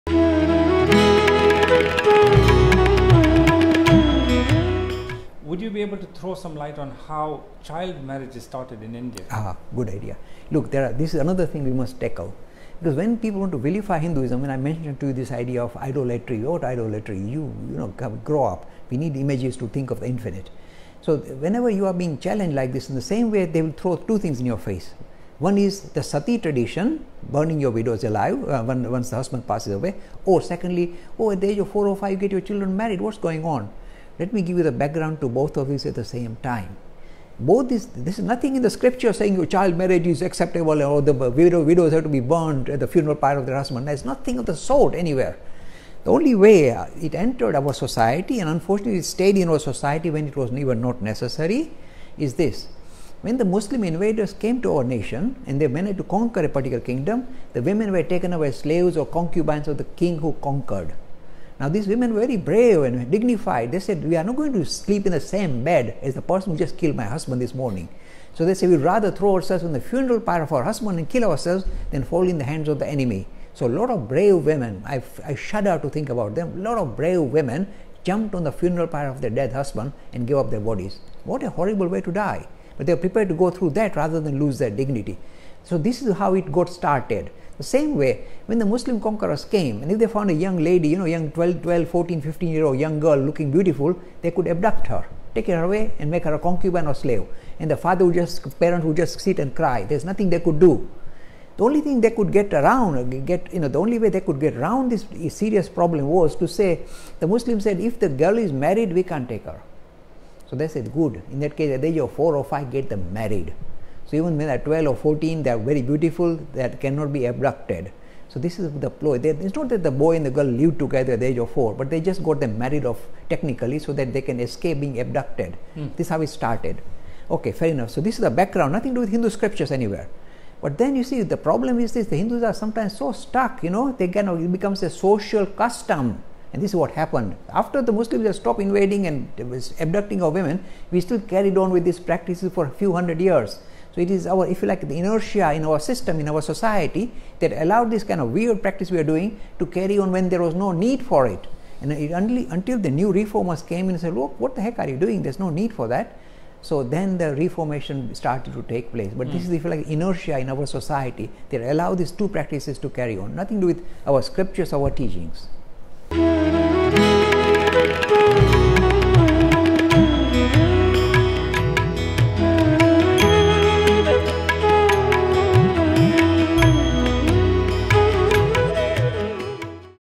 What is the background to child Marriages and Sati traditions? | Jay Lakhani | Hindu Academy
What is the background to child Marriages and Sati traditions?

for more interesting information on Hinduism by Jay Lakhani.